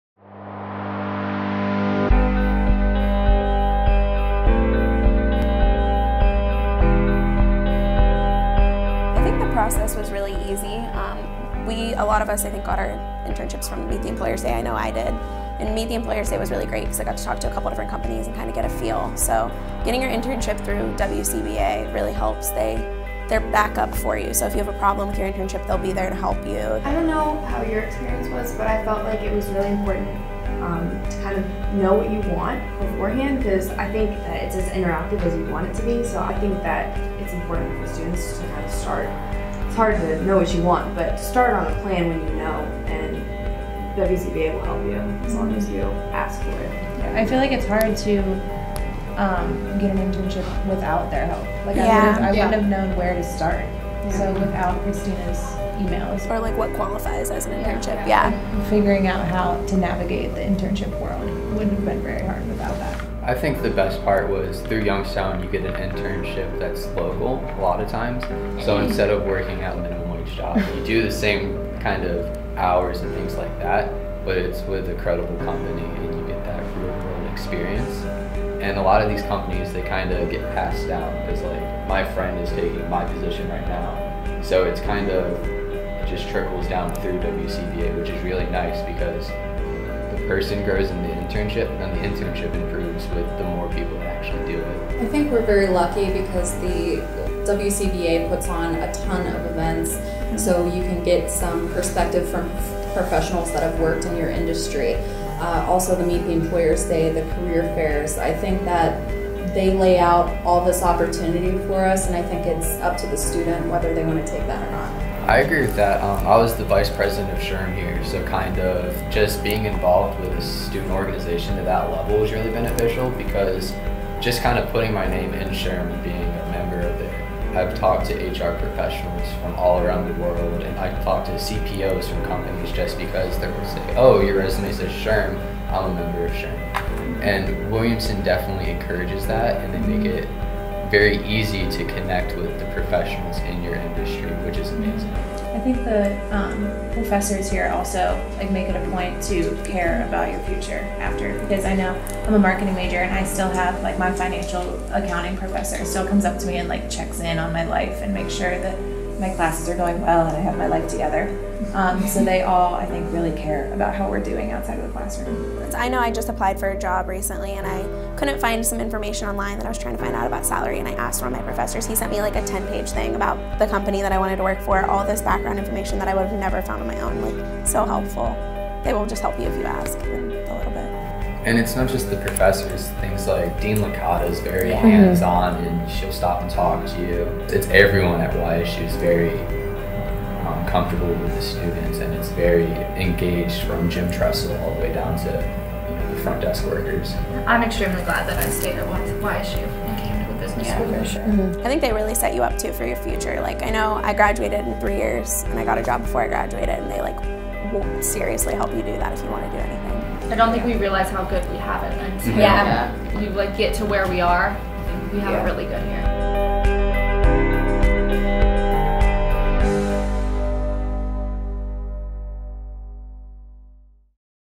The WCBA Internship Experience
Learn about the internship opportunities and experiences of WCBA students.